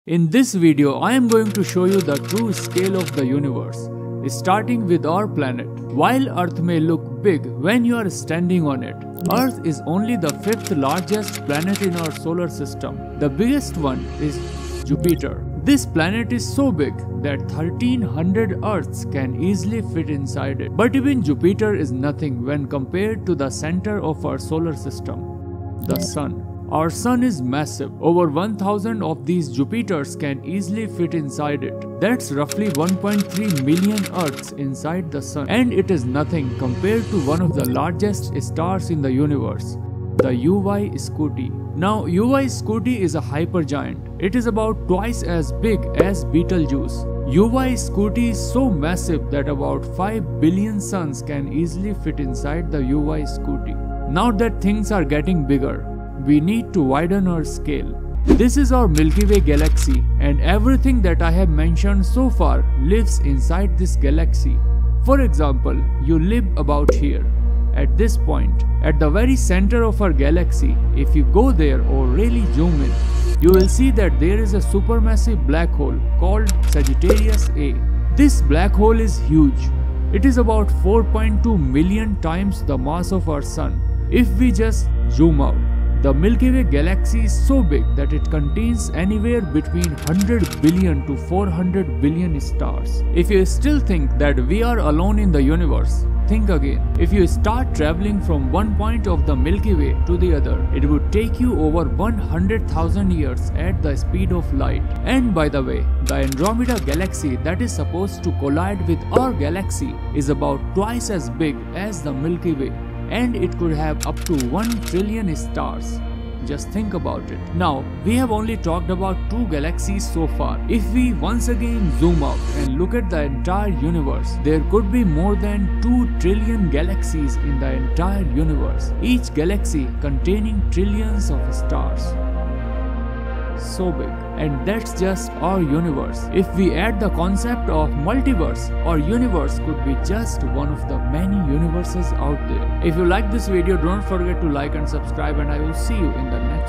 True Scale of The Universe!
In this video we're going to look at the scale of the universe.